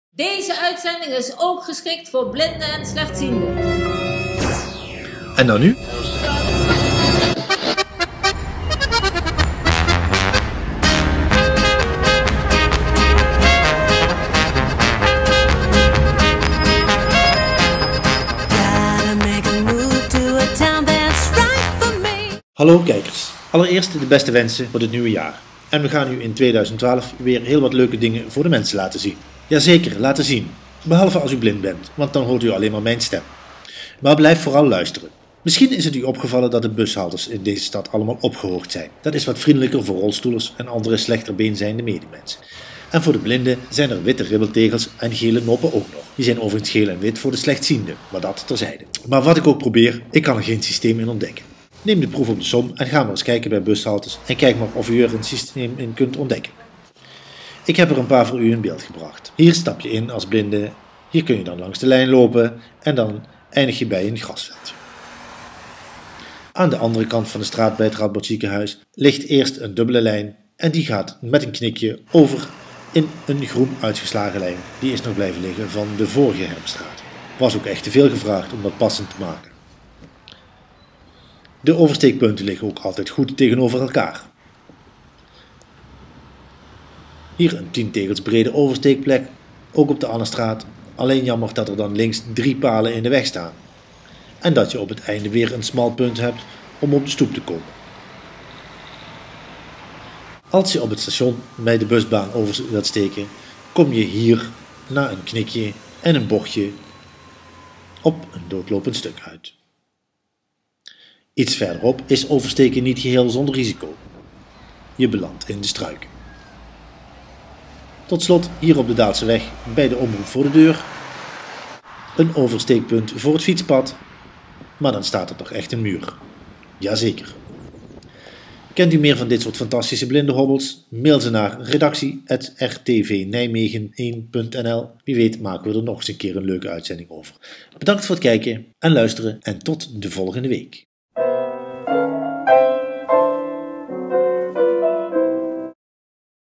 Hier ga ik wel over!
Alle bushaltes in de stad zijn voorzien van gele en witte ribbeltegels, voor blinden en slechtzienden. Maar er zit, voor zover ik heb kunnen ontdekken, geen enkel systeem in de bestrating. Kijk hier naar een paar varianten en de gevolgen. Je zal maar blind zijn in Nijmegen en met de bus moeten reizen...